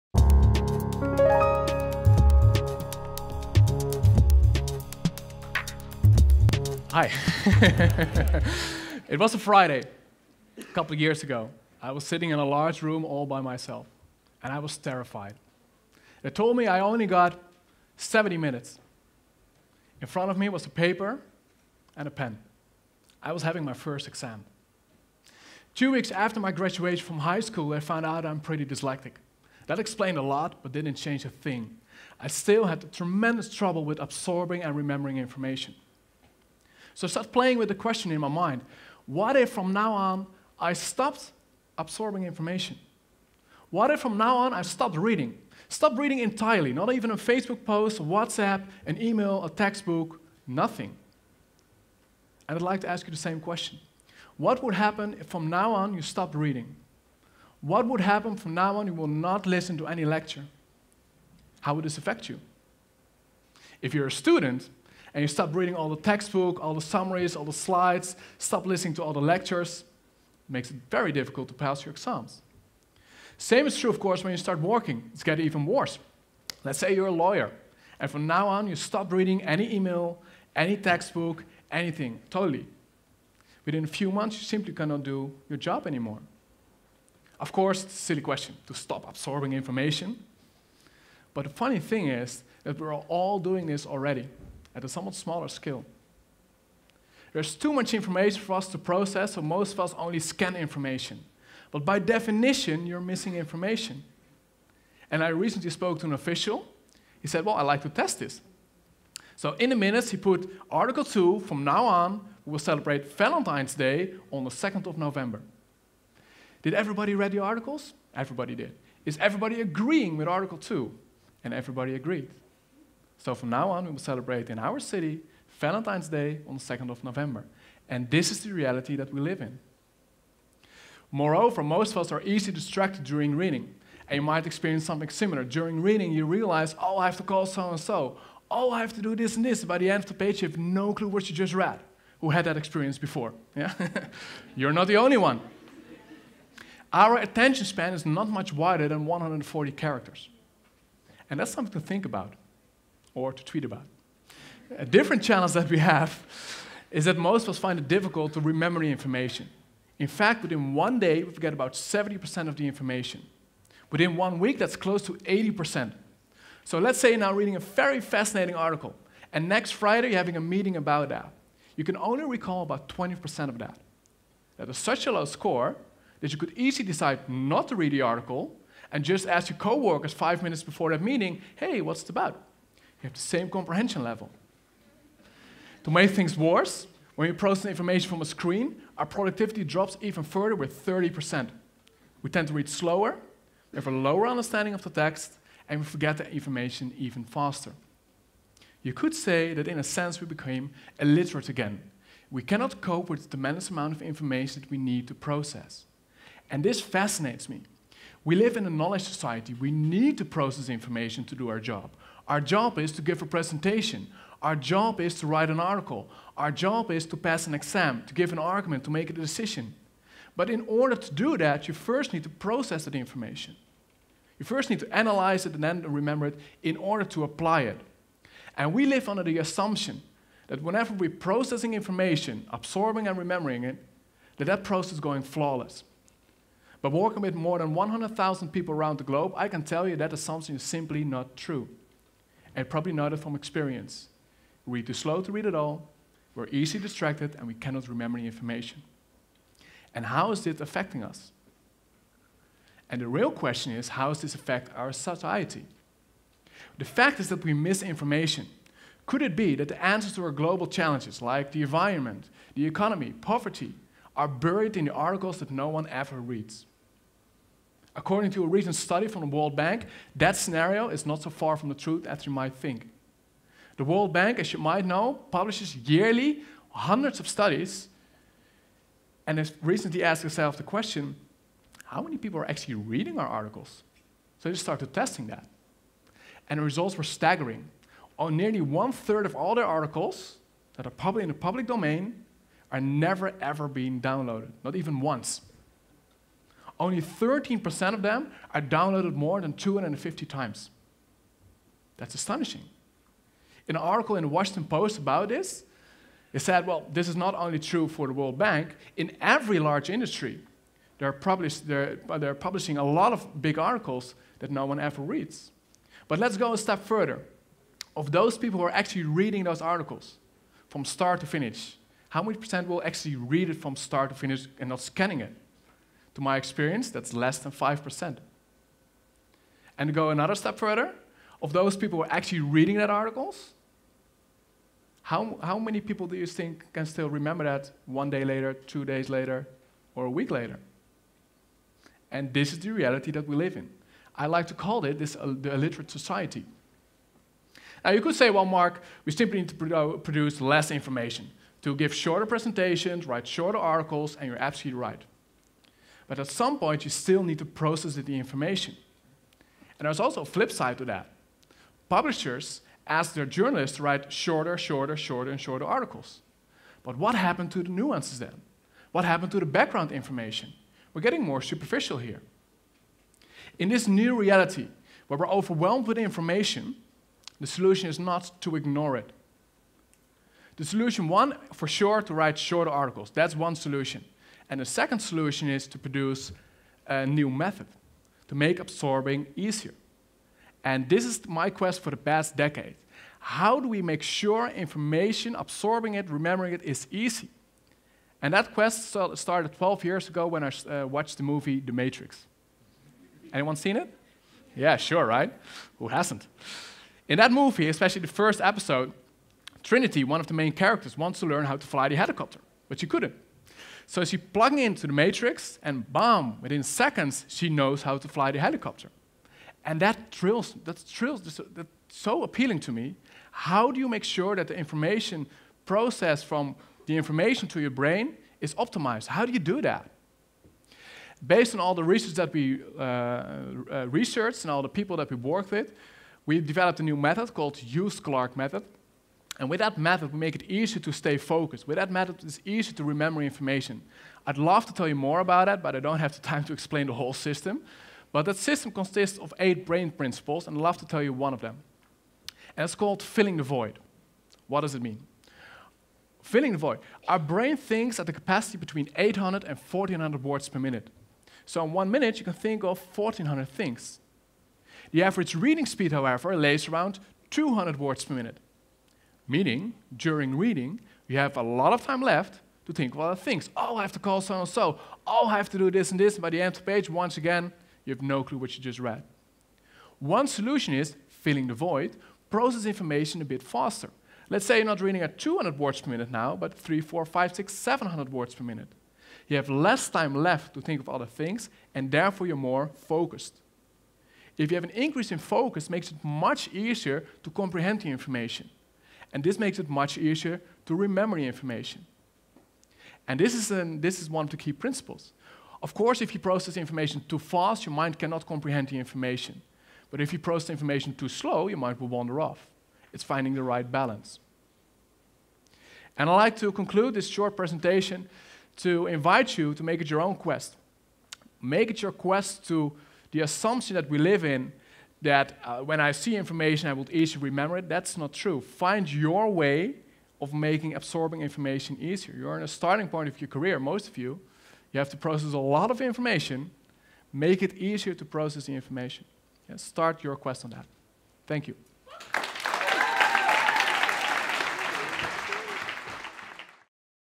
The illiterate society | Mark Tigchelaar | TEDxYouth@Maastricht
People with Dylexia suffer. With the help of Mark Tigchelaar and the idea behind speed reading leads to people being able to train their brains to read faster and retain what they just read

Mark Tichelaar is an expert in speed-reading. He has trained over 70,000 people to gain more from their brain in areas such as reading faster, showing to stay concentrated, how to remember information longer, etc. Through working with these people, he has been able to draw a conclusion. In a sense, the human race is illiterate again. People tend to half read, skim or sometimes even entirely skip entire articles. Nowadays, people are easily distracted when reading which leads to people reading entire pages from books without having processed a single word they read. We are also unable to remember information. In fact, on average we forget about 70% within 1 day! His talk will be called, “The illiterate society” in which he will use his 10 years of experience in Neuroscience and software he’s developed helping people to absorb information much more efficiently.